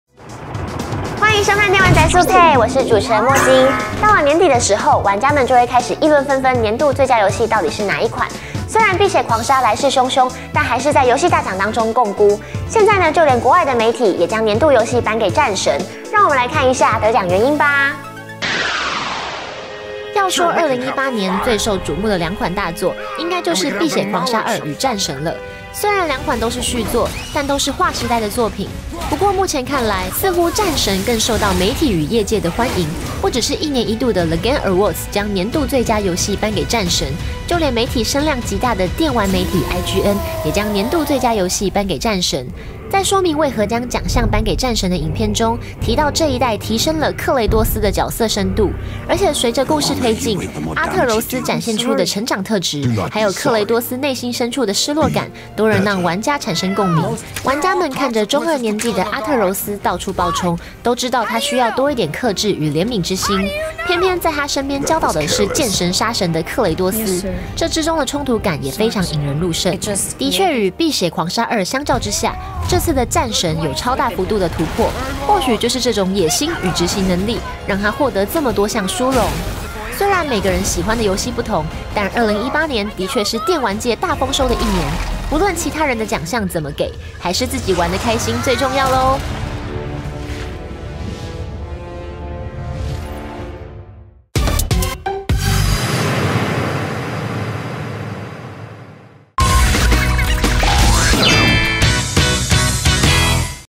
國外媒體頒發年度遊戲，奎爺連莊!《碧血狂殺2》再次槓龜_電玩宅速配20190102
要說2018年最受矚目的兩款大作，應該就是《碧血狂殺2》與《戰神》了。雖然兩款都是續作，但都是劃時代的作品。不過目前看來，似乎《戰神》更受到媒體與業界的歡迎。不只是一年一度的The Game Awards將年度最佳遊戲頒給戰神，就連媒體聲量極大的電玩媒體IGN，也將年度最佳遊戲頒給《戰神》。

在說明為何將獎項頒給《戰神》的影片中，提到這一代提昇了克雷多斯的角色深度。而且隨著故事推進，阿特柔斯展現出的成長特質，還有克雷多斯內心深處的失落感，都能讓玩家產生共鳴。玩家們看著中二年紀的阿特柔斯到處暴衝，都知道他需要多一點克制與憐憫之心，偏偏在他身邊教導的是見神殺神的克雷多斯，這之中的衝突感也非常引人入勝。

的確與《碧血狂殺2》相較之下，這次的《戰神》有超大幅度的突破，或許就是這種野心與執行能力，讓它獲得這麼多項殊榮。雖然每個人喜歡的遊戲不同，但2018年的確是電玩界大豐收的一年。不論其他人的獎項怎麼給，還是自己玩得開心最重要囉！

（C）Sony Interactive Entertainment